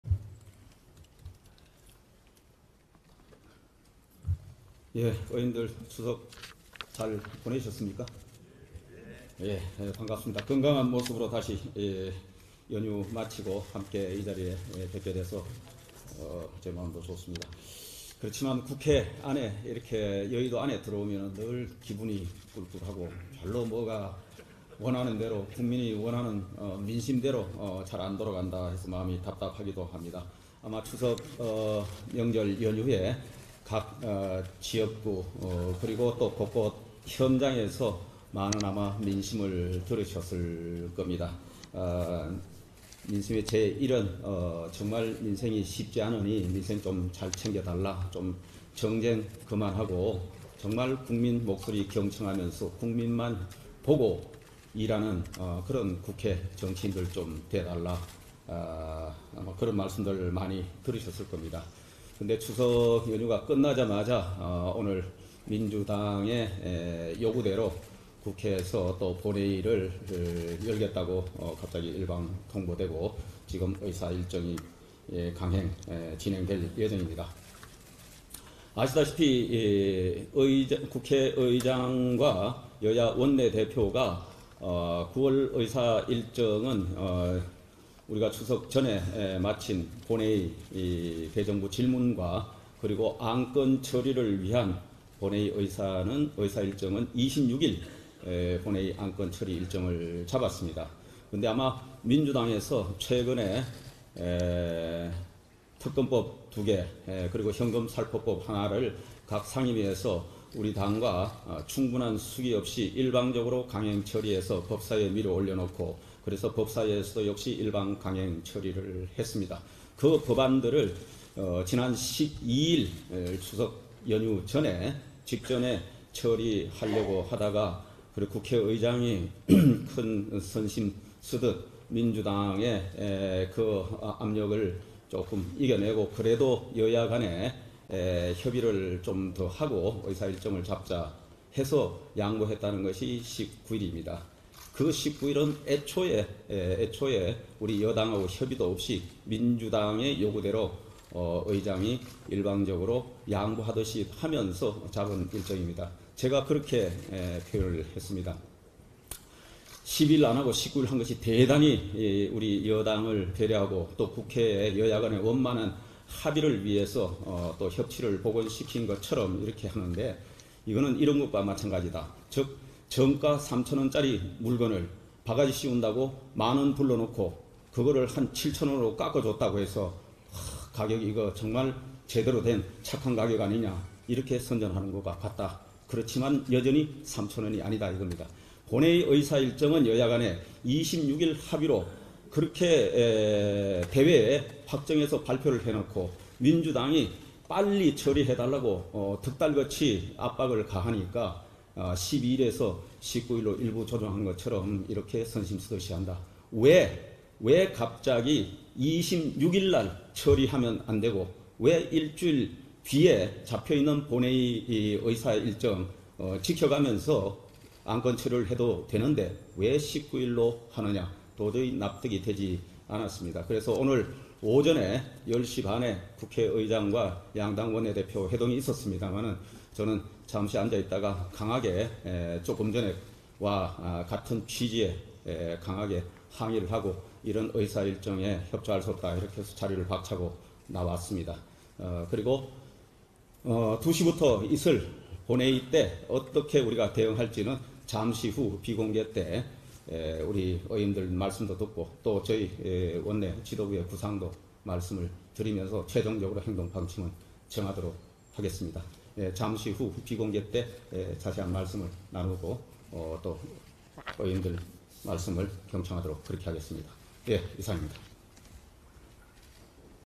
의원총회장에 선 추경호 "추석 연휴 끝나자마자 민주당이 원하는대로 본회의 강행...비공개때 대응 얘기 나누자"
국민의힘 비상의원총회 (24.09.19)